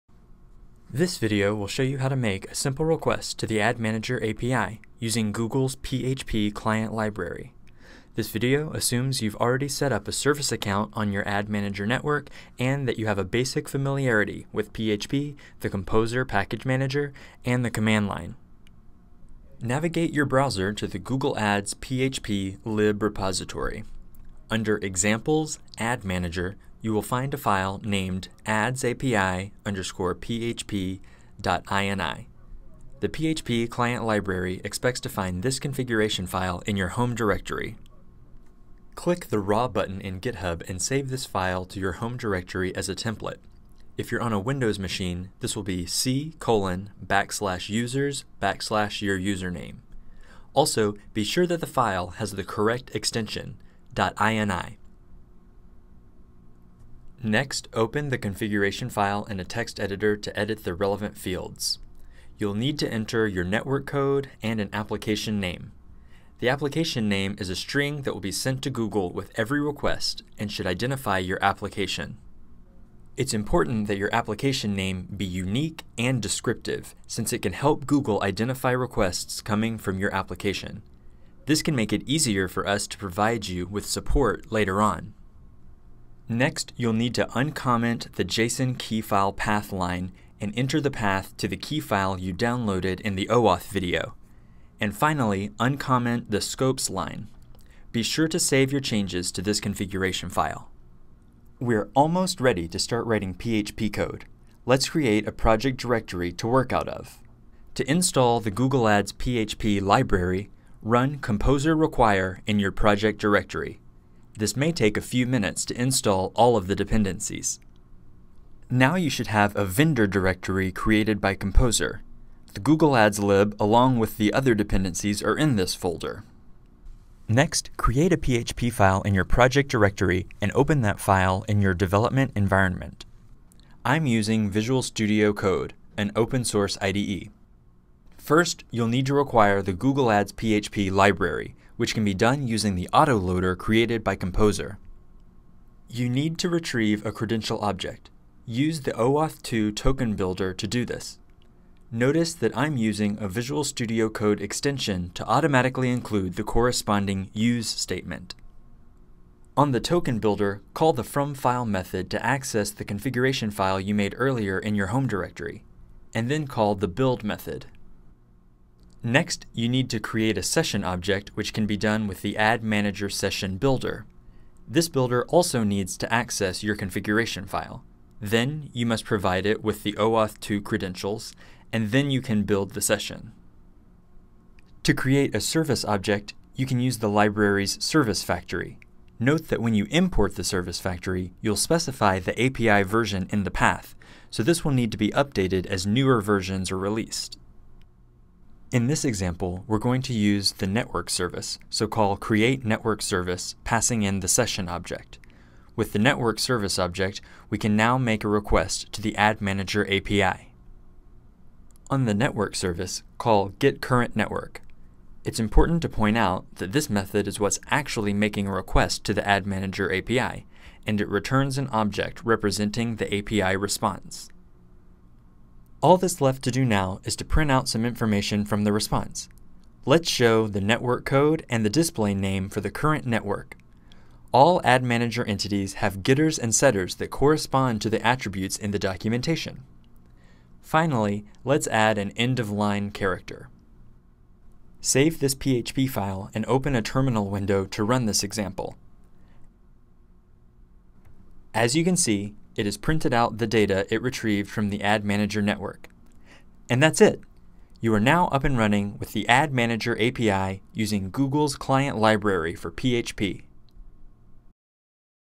Making Requests to Google Ad Manager API with PHP
In this video, we'll cover how to make a basic request to the Google Ad Manager API with Google's client library for PHP.